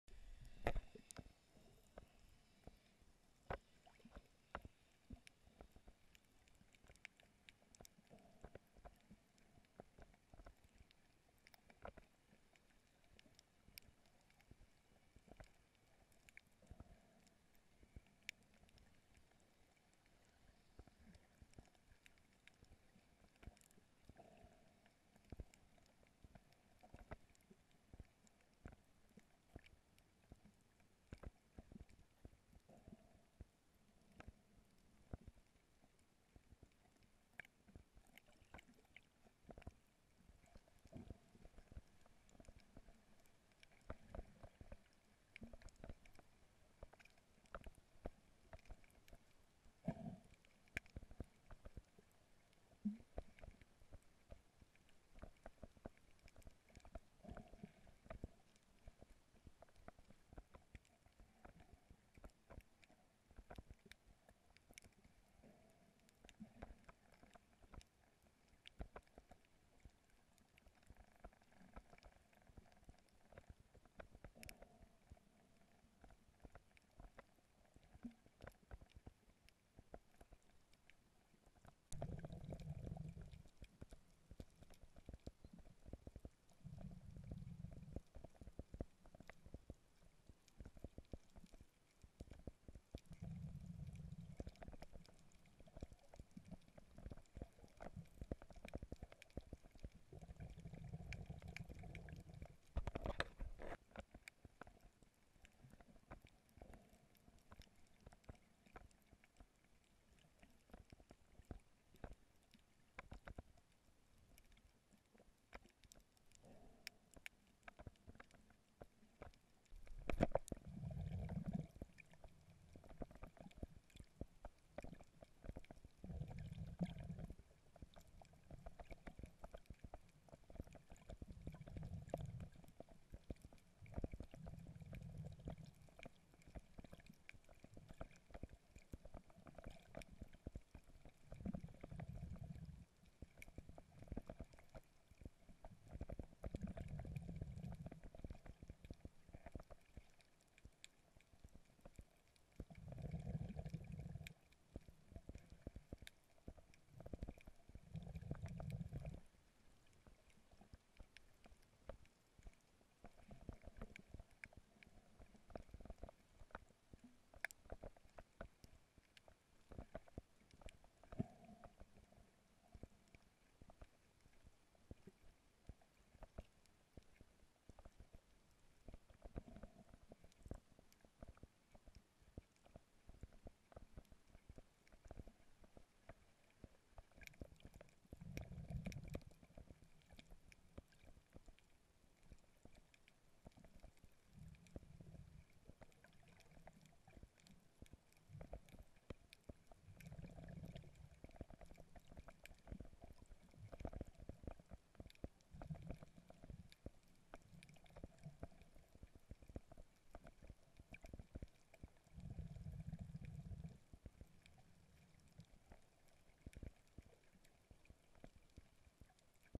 Snorkeling. Reefs Sharks bay. Red sea. Egypt
Рифы между отелями Айланд вью и Савой. snorkeling Sharm-El-Sheikh Egypt fish Египет карантин Шарм-эль-Шейх
На 1-26 стайка скумбрии утром охотится на планктон. Эти рыбы разгоняются и плывут с широко  открытой пастью.) Под водой это шикарно смотрится.